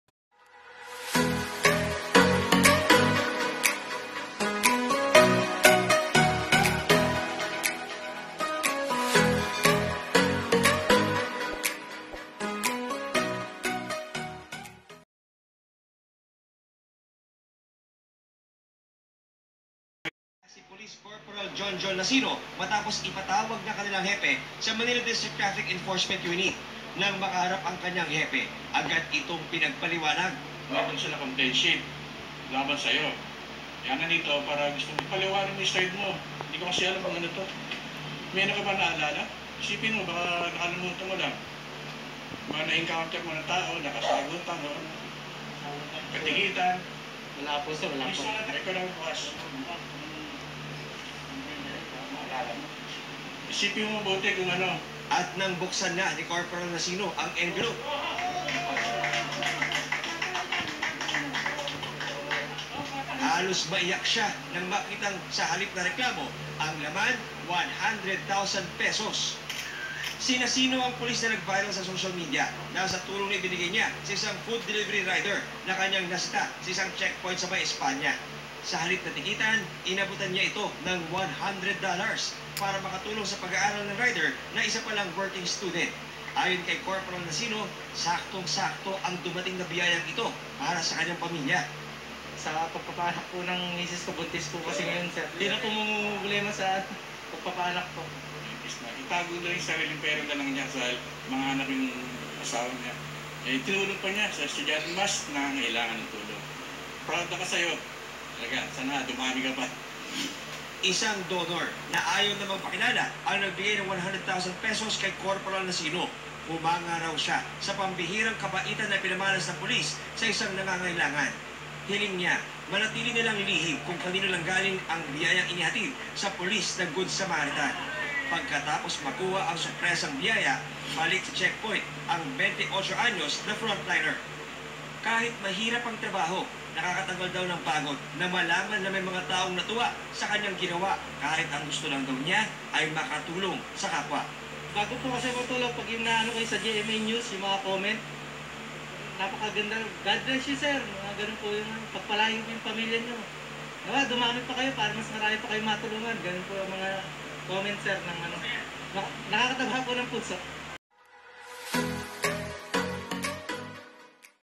THE GOOD SAMARITAN POLICE GET REWARD FOR HELPING DELIVERY BOY. please subscribe
after giving $100 to the delivery boy ..
he get reward 100,000 from anonymous person ..